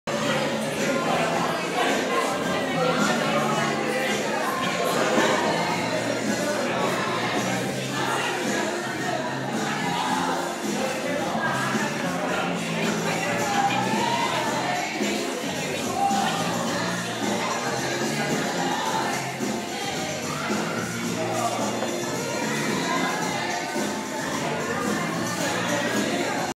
4 May 2010 - Beginner Routine
This is a video of the routine we taught for Beginners on 4 May 2010!